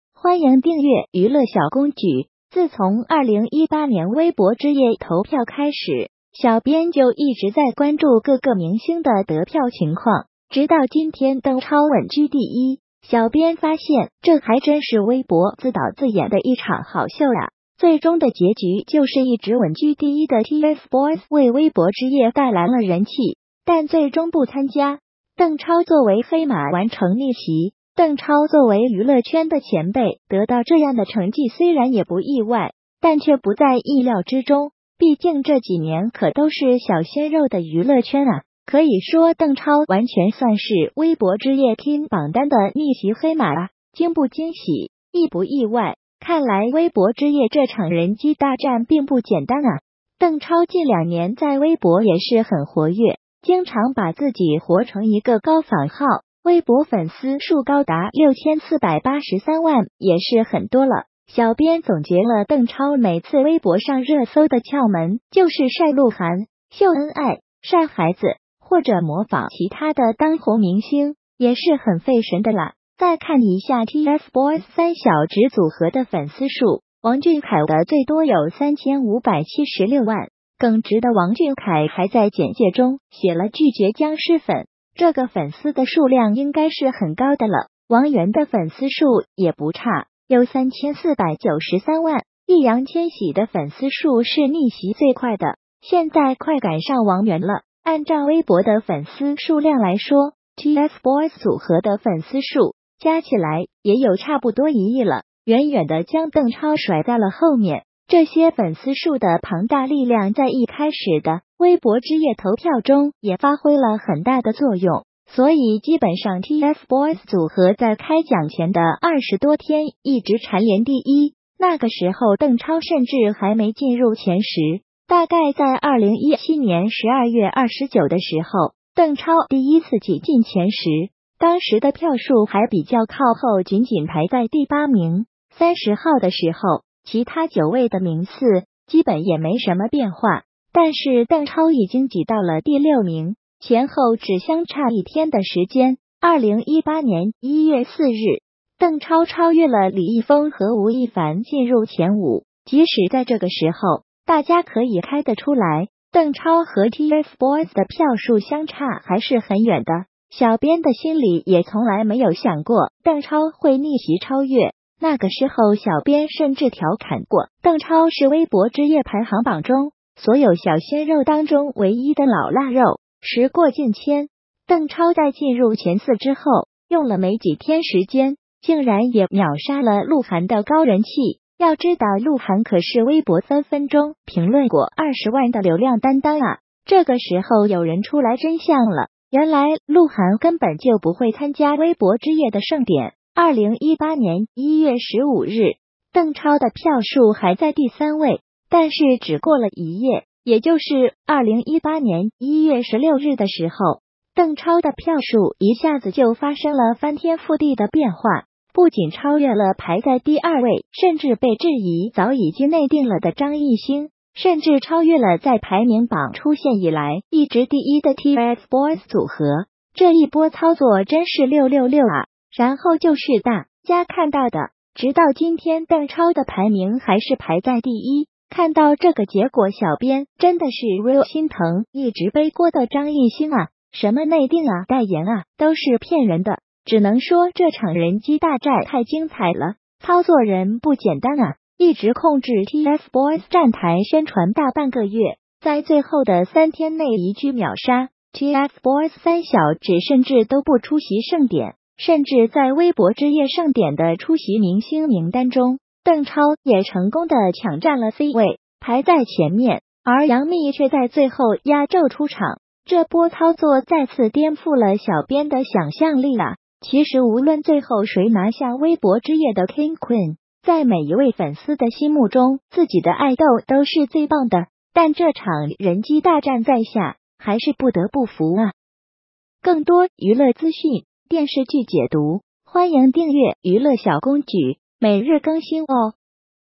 黑幕？ TFBOYS三人票數不敵鄧超一人？微博之夜這場人機大戰不簡單
自從2018年微博之夜投票開始，小編就一直在關注各個明星的得票情況，直到今天鄧超穩居第一，小編髮現這還真是微博自導自演的一場好秀啊，最終的結局就是一直穩居第一的TFboys為微博之夜帶來了人氣，但最終不參加，鄧超作為黑馬完成逆襲。鄧超作為娛樂圈的前輩得到這樣的成績雖然也不意外，但卻不在意料之中，畢竟這幾年可都是小鮮肉的娛樂圈啊，可以說鄧超完全算是微博之夜King榜單的逆襲黑馬啊，驚不驚喜？意不意外？看來微博之夜這場人機大戰並不簡單啊。鄧超近兩年在微博也是很活躍，經常把自己活成一個高仿號，微博粉絲數高達6483萬也是很多了。小編總結了鄧超每次微博上熱搜的竅門就是曬鹿晗、秀恩愛、曬孩子、或者模仿其他的當紅明星，也是很費神的啦。再看一下TFboys三小只組合的粉絲數，王俊凱的最多有3576萬，耿直的王俊凱還在簡介中寫了拒絕殭屍粉，這個粉絲的數量應該是很高的了。王源的粉絲數也不差，有3493萬，易烊千璽的粉絲數是逆襲最快的，現在快趕上王源了。按照微博的粉絲數量來說，TFboys組合的粉絲數加起來也有差不多1億了，遠遠地將鄧超甩在了後面，這些粉絲數的龐大力量在一開始的微博之夜投票中也發揮了很大的作用，所以基本上TFboys組合在開獎前的二十多天一直蟬聯第一，那個時候鄧超甚至還沒進入前十。大概在2017年12月29的時候，鄧超第一次擠進前十，當時的票數還比較靠後僅僅排在第八名，30號的時候其他九位的名次基本也沒什麼變化，但是鄧超已經擠到了第六名，前後只相差一天的時間，2018年1月4日，鄧超超越了李易峰和吳亦凡進入前五。即使在這個時候，大家可以開的出來，鄧超和TFboys的票數相差還是很遠的，小編的心裡也從來沒有想過鄧超會逆襲超越，那個時候小編甚至調侃過鄧超是微博之夜排行榜中所有小鮮肉當中唯一的老臘肉。時過境遷，鄧超在進入前四之後用了沒幾天時間，竟然也秒殺了鹿晗的高人氣，要知道鹿晗可是微博分分鐘評論過二十萬的流量擔當啊，這個時候有人出來真相了，原來鹿晗根本就不會參加微博之夜的盛典。 2018年1月15日，鄧超的票數還在第三位，但是只過了一夜，也就是2018年1月16日的時候，鄧超的票數一下子就發生了翻天覆地的變化，不僅超越了排在第二位甚至被質疑早已經內定了的張藝興，甚至超越了在排名榜出現以來一直第一的TFboys組合，這一波操作真是666啊。然後就是大家看到的，直到今天鄧超的排名還是排在第一，看到這個結果小編真的是real心疼一直背鍋的張藝興啊，什麼內定啊代言啊都是騙人的，只能說這場人機大戰太精彩了，操作人不簡單啊。一直控制TFboys站台宣傳大半個月，在最後的三天內一句秒殺，TFboys三小只甚至都不出席盛典。甚至在微博之夜盛典的出席明星名單中，鄧超也成功的搶占了C位，排在前面，而楊冪卻在最後壓軸出場，這波操作再次顛覆了小編的想像力啊，其實無論最後誰拿下微博之夜的King/Queen，在每一位粉絲的心目中自己的愛豆都是最棒的，但這場人機大戰在下還是不得不服啊。